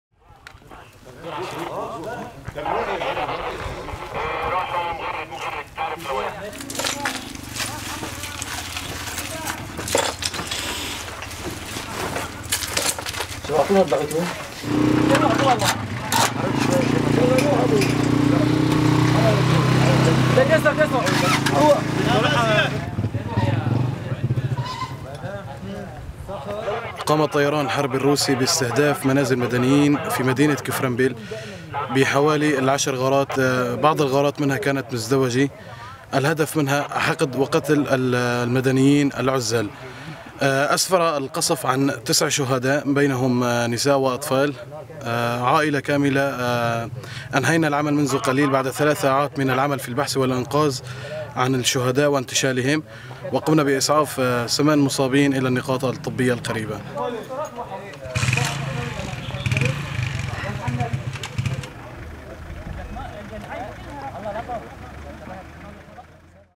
ضحايا بقصف جوي روسي على مدينة كفرنبل بإدلب
قتل 9 أشخاص  بينهم نساء وأطفال  الاثنين 20 أيار 2019، وجرح 8 آخرون، إثر غارات لطائرات حربية روسية على الأحياء السكنية في مدينة كفرنبل قرب إدلب شمالي سوريا.

وقال عضو المكتب الإعلامي للدفاع المدني في المدينة رامي سويد، إن عشر غارات شنتها الطائرات الحربية الروسية على الأحياء السكنية في مدينة كفرنبل، ما أسفر عن مقتل 9 أشخاص بينهم نساء وأطفال وجرح 8 آخرين عمل الدفاع المدني على إسعافهم للنقاط الطبية.

 وتتعرض محافظة إدلب وشمال مدينة حماة  لقصف جوي ومدفعي وصاروخي متكرر من قبل قوات النظام وروسيا، ما يسفر عن قتلى وجرحى بين المدنيين، وعشرات آلاف النازحين، رغم أنها مشمولةبالاتفاق الروسي التركي الذي يتضمن إيقاف القصف على المنطقة.